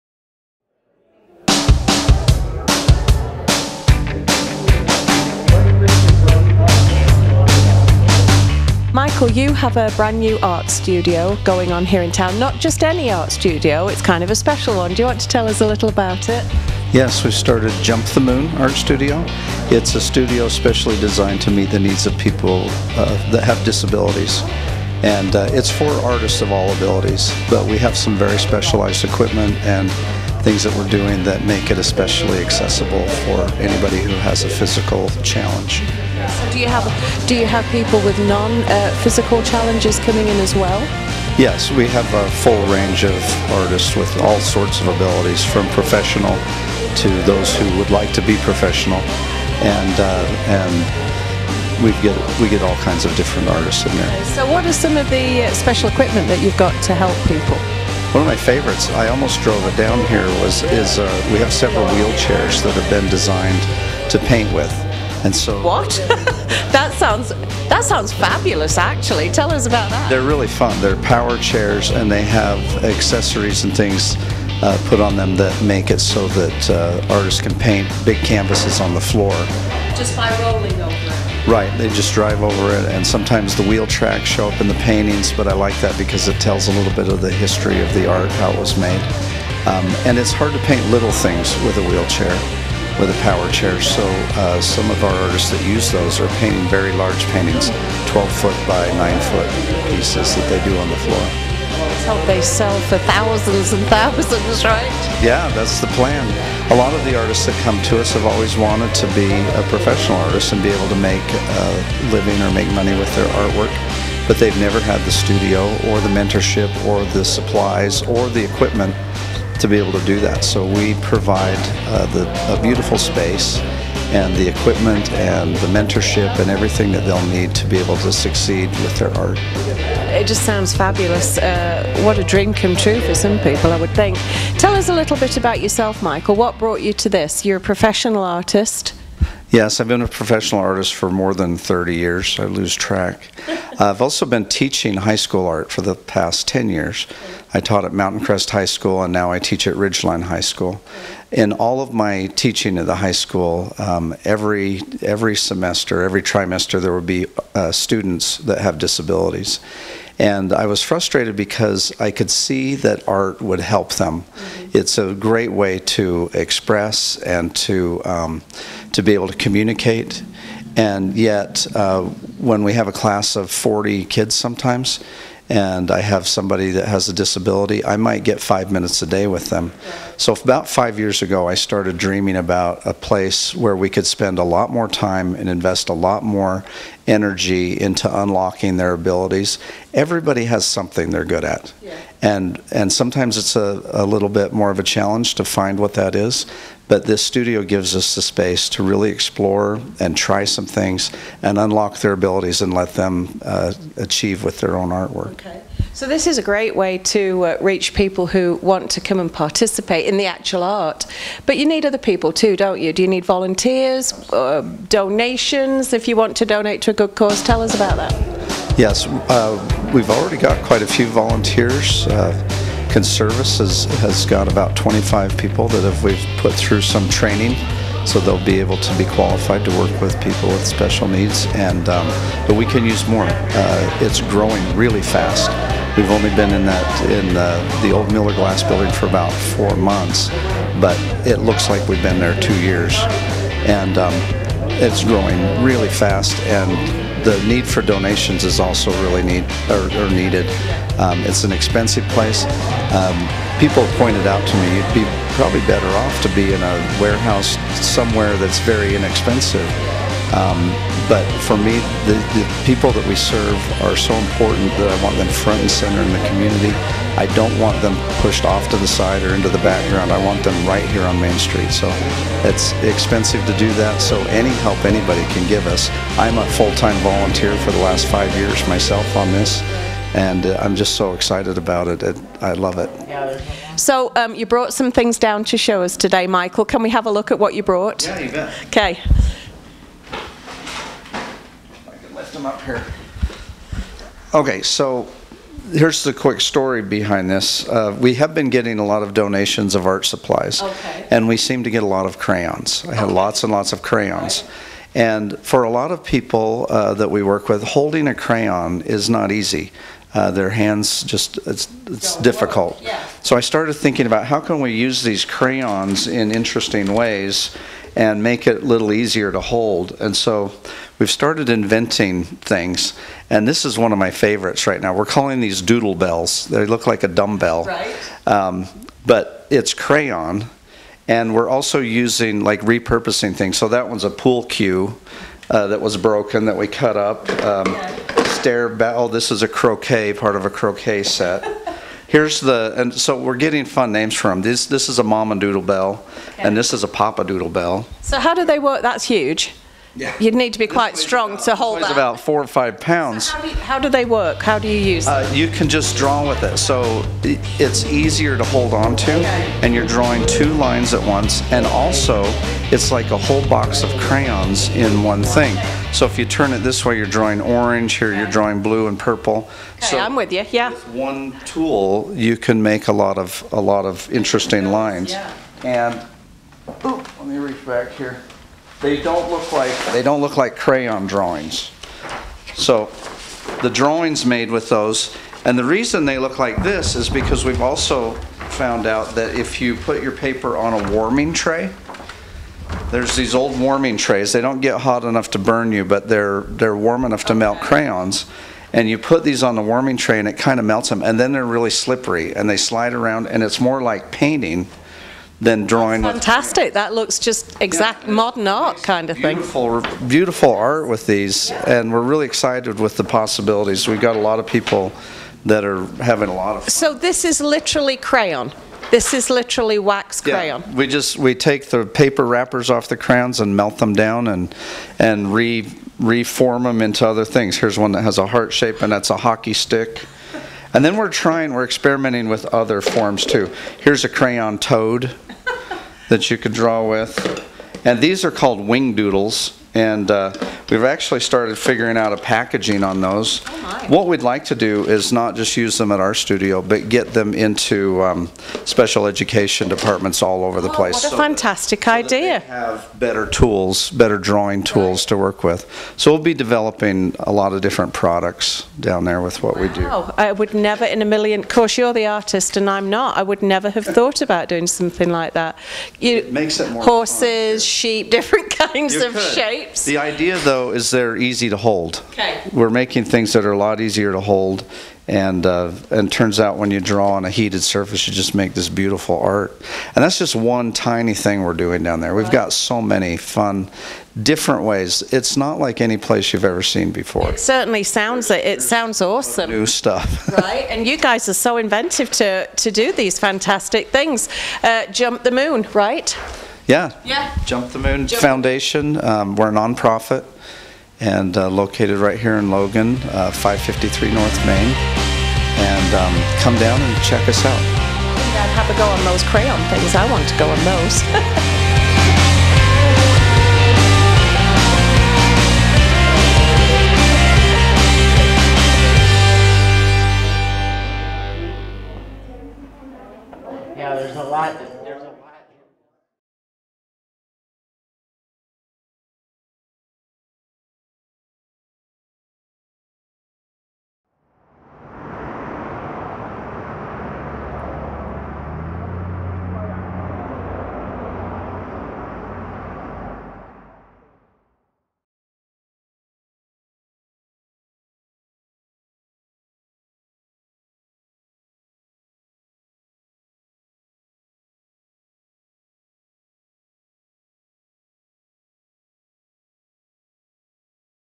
Jump The Moon Studios - Art Gallery and Vocational Arts program - Mission statement
Jump The Moon Studios is an Art Gallery and Vocational Arts program helping artists with disabilities to make, market, and earn income from their original, compelling artworks

Established in 2017, Jump The Moon believes that artists with disabilities are vital to a robust arts landscape and should be afforded the opportunity to pursue art as a vocation and career. Jump the Moon works with artists that experience a broad range of developmental and cognitive disabilities as well as mental health challenges including autism, Down syndrome, traumatic brain injury, schizophrenia and bipolar disorder, among others.

Founder Michael Bingham
553 North Main St